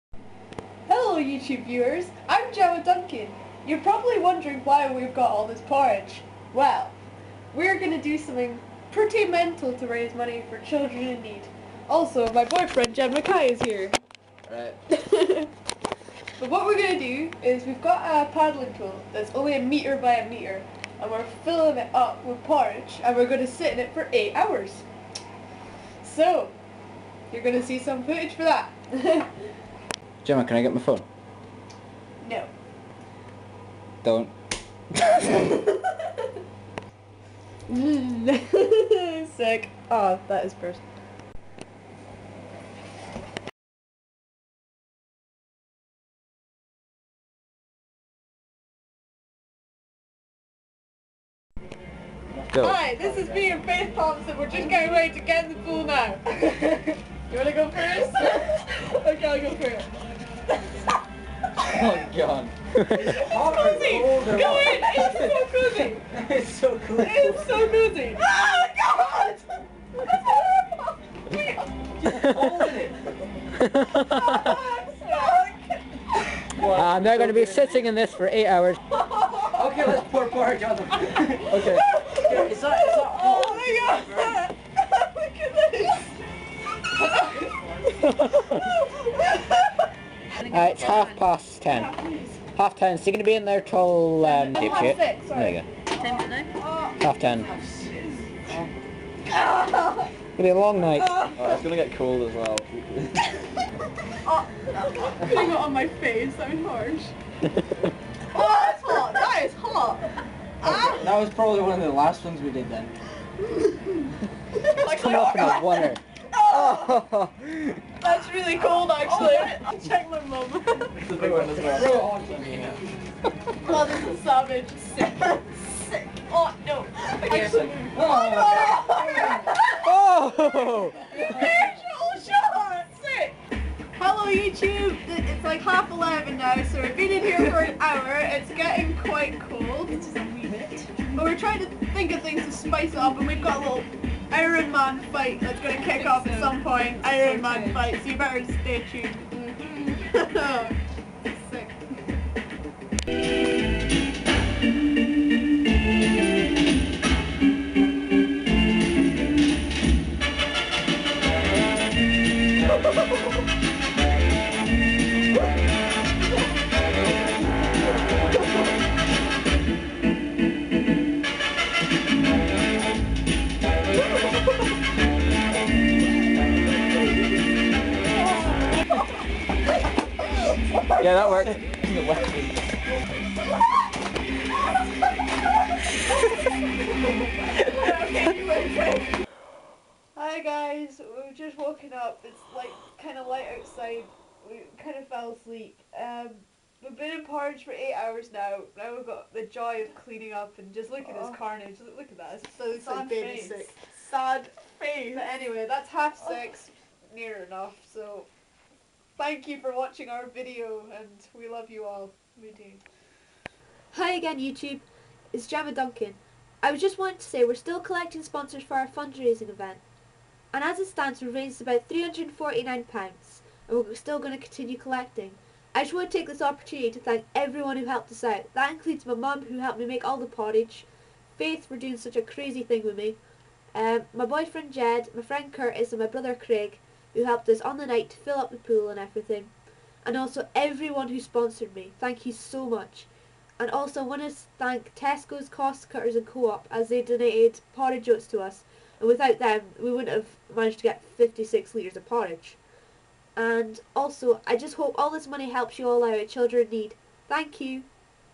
Porridge Fundraiser Children in Need 2010
Watch as Faith and I prepare and in Porridge for 8 hours! :D